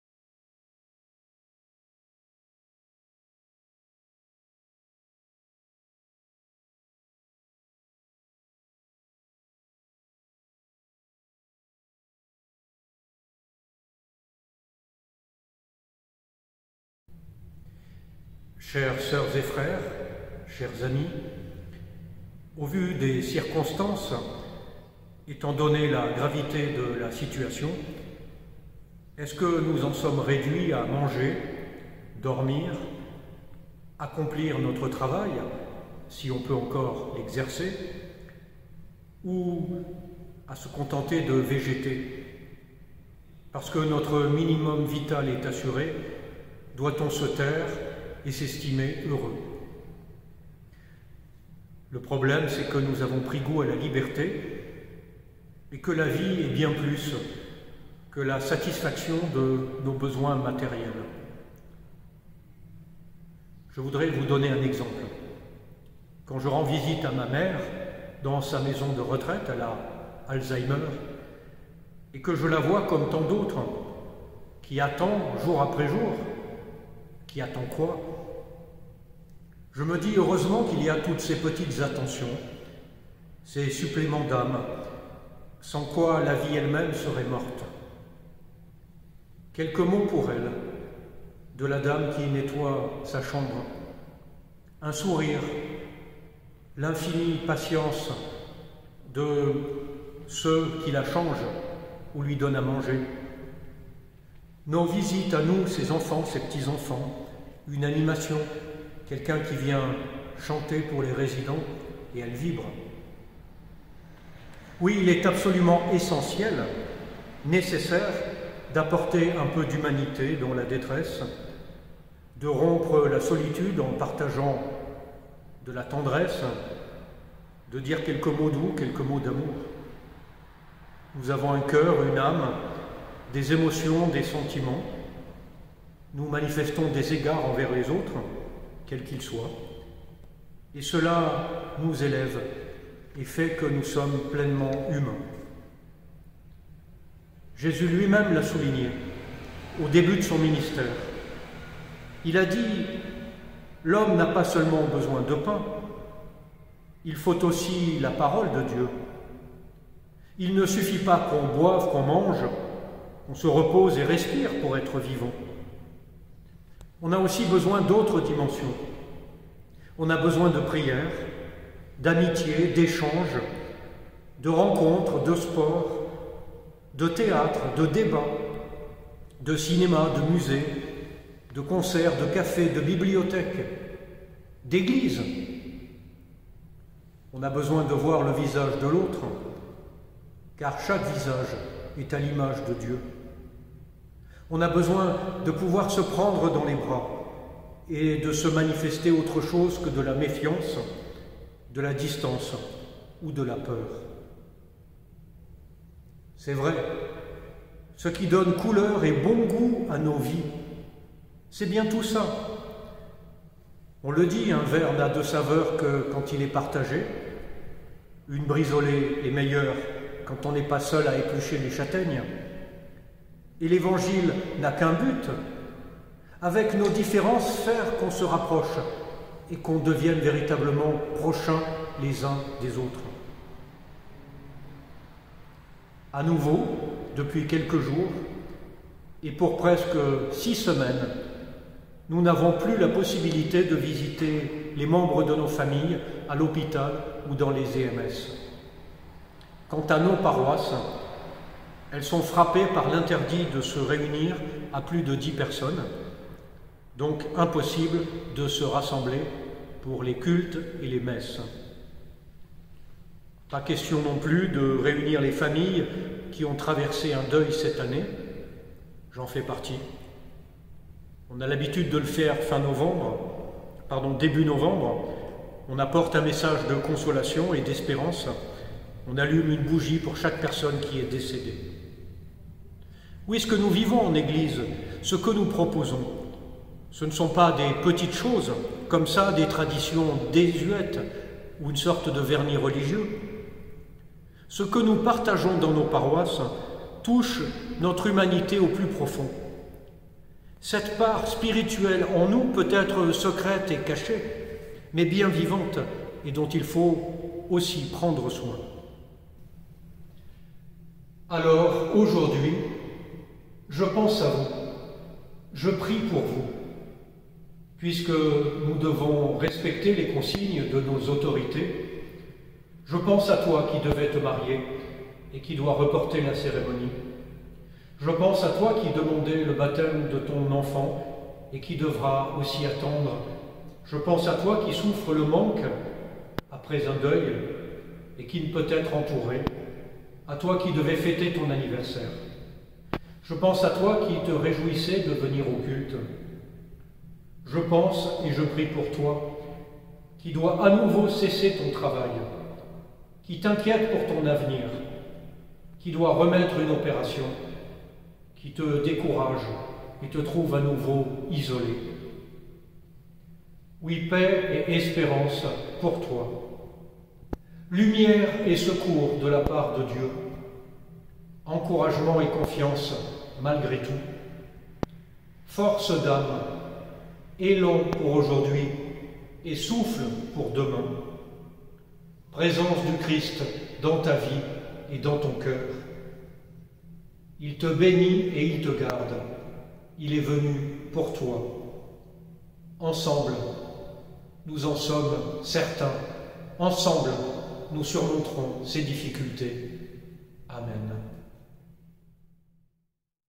Message de Pierre Boismorand
Le pasteur Pierre Boismorand vous adresse un message en cette période de nouvelles mesures.